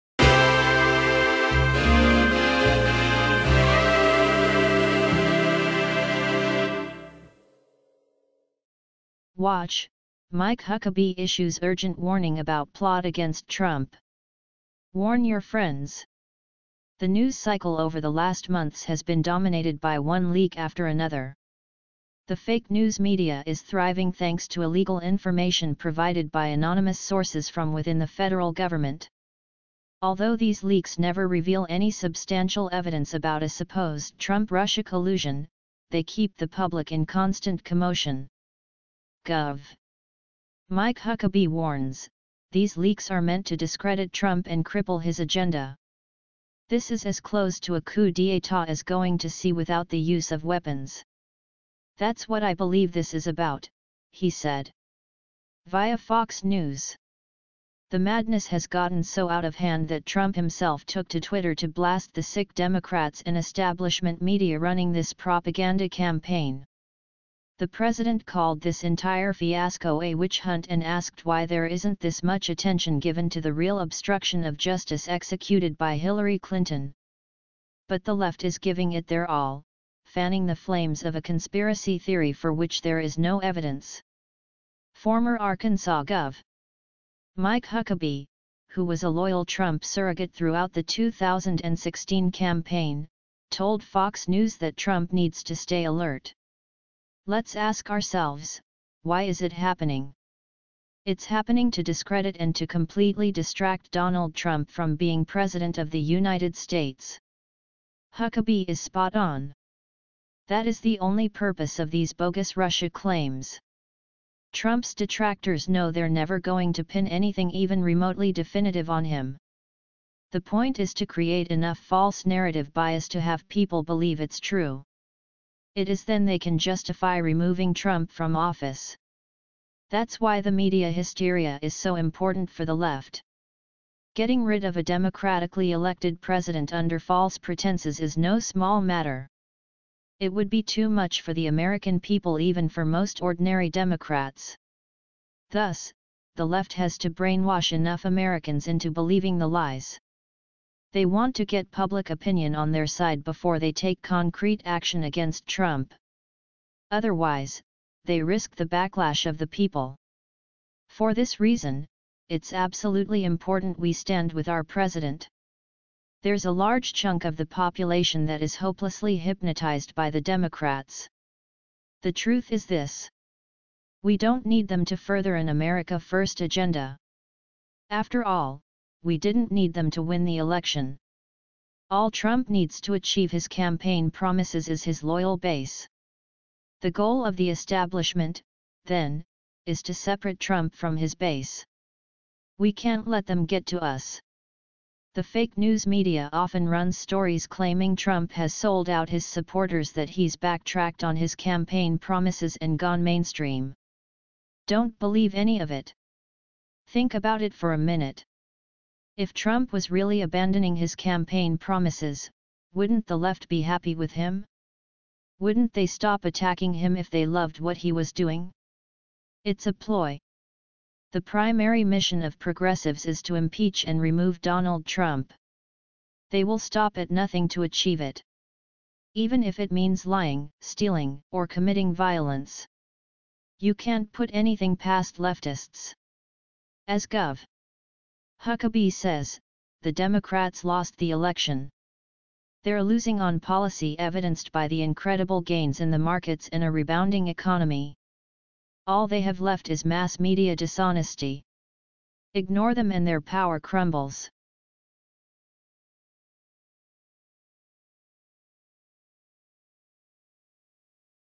WATCH: Mike Huckabee Issues Urgent Warning About Plot Against Trump. Warn Your Friends.
The news cycle over the last months has been dominated by one leak after another. The fake news media is thriving thanks to illegal information provided by anonymous sources from within the federal government.
Although these leaks never reveal any substantial evidence about a supposed “Trump-Russia collusion,” they keep the public in constant commotion. Gov. Mike Huckabee warns: these leaks are meant to discredit Trump and cripple his agenda. “This is as close to a coup d’état as going to see without the use of weapons. That’s what I believe this is about,” he said. (via Fox News)
The madness has gotten so out of hand that Trump himself took to Twitter to blast the SICK Democrats and establishment media running this propaganda campaign. The President called this entire fiasco a “witch hunt” and asked why there isn’t this much attention given to the real obstruction of justice executed by Hillary Clinton.
But the Left is giving it their all, fanning the flames of a conspiracy theory for which there is no evidence. Former Arkansas Gov. Mike Huckabee, who was a loyal Trump surrogate throughout the 2016 campaign, told FOX News that Trump needs to stay alert.
“Let’s ask ourselves: Why is it happening? It’s happening to discredit and to completely distract Donald Trump from being President of the United States.”
Huckabee is spot-on. That is the only purpose of these bogus Russia claims. Trump’s detractors know they’re never going to pin anything even remotely definitive on him. The point is to create enough false narrative bias to have people believe it’s true. It is then they can justify removing Trump from office.
That’s why the media hysteria is so important for the Left. Getting rid of a democratically-elected President under false pretenses is no small matter. It would be too much for the American people — even for most ordinary Democrats.
Thus, the Left has to brainwash enough Americans into believing the lies. They want to get public opinion on their side before they take concrete action against Trump. Otherwise, they risk the backlash of the people.
For this reason, it’s absolutely important we stand with our president! There’s a large chunk of the population that is hopelessly hypnotized by the Democrats. The Truth is this. We don’t need them to further an America First agenda. After all, we didn’t need them to win the election. All Trump needs to achieve his campaign promises is his loyal base. The goal of the establishment, then, is to separate Trump from his base.
We can’t let them get to us! The fake news media often runs stories claiming Trump has “sold out” his supporters — that he’s “backtracked” on his campaign promises and gone “mainstream.” Don’t believe any of it. Think about it for a minute. If Trump was really abandoning his campaign promises, wouldn’t the Left be happy with him? Wouldn’t they stop attacking him if they loved what he was doing?
It’s a ploy! The primary mission of progressives is to impeach and remove Donald Trump. They will stop at nothing to achieve it. Even if it means lying, stealing, or committing violence. You can’t put anything past Leftists.
As Gov. Huckabee says, the Democrats lost the election. They’re losing on policy evidenced by the incredible gains in the markets and a rebounding economy. All they have left is mass media dishonesty. Ignore them and their power crumbles!